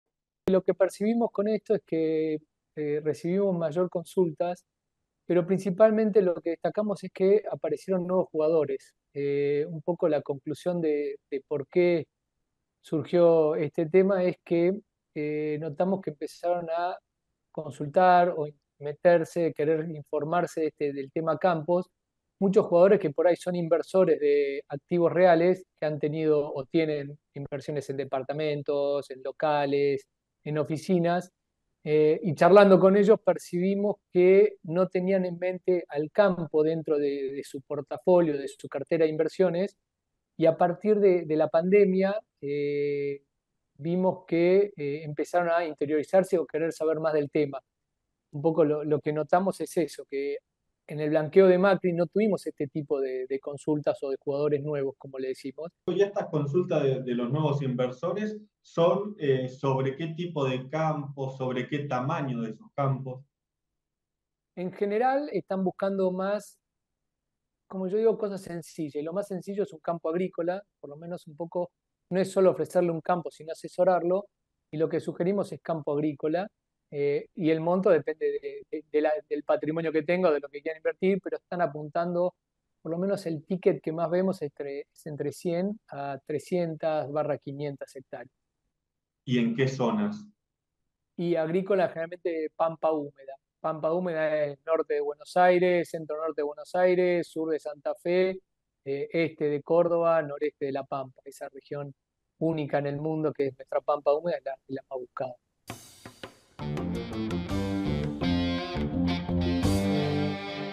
Federico Nordheimer – Consultor Inmobiliario Rural - 21-10-2024 - EDICIÓN CENTRAL
Aseguran que el blanqueo impulsa la compra - venta de campos